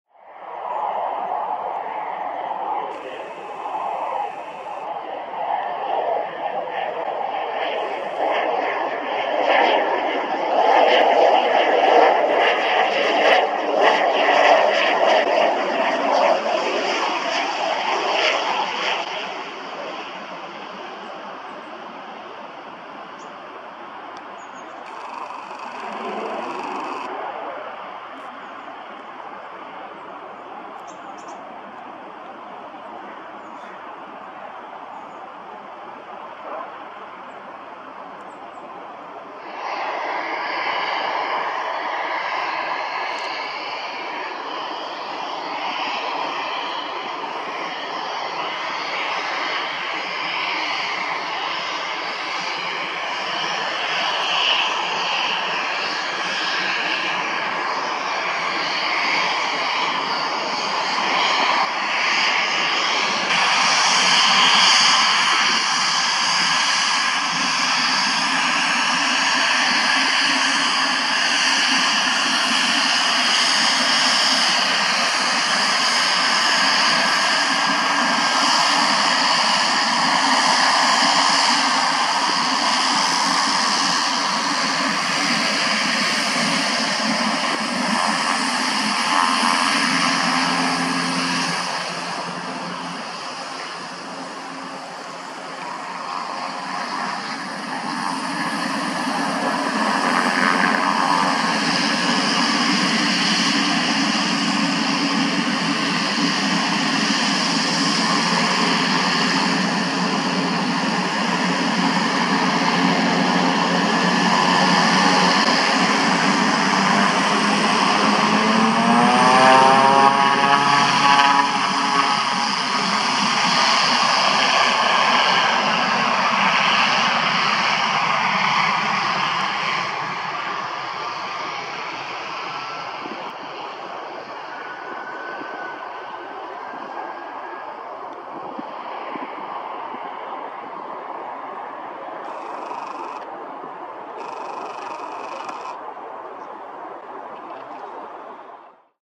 Turboprop in the Mist | Fly 7 Pilatus PC-12 NGX OH-IBZ at Cambridge
Fly 7 Pilatus PC-12 NGX OH-IBZ at Cambridge on a foggy afternoon of January 11, 2024, arriving at 14:10 UTC and departing at 15:25 UTC.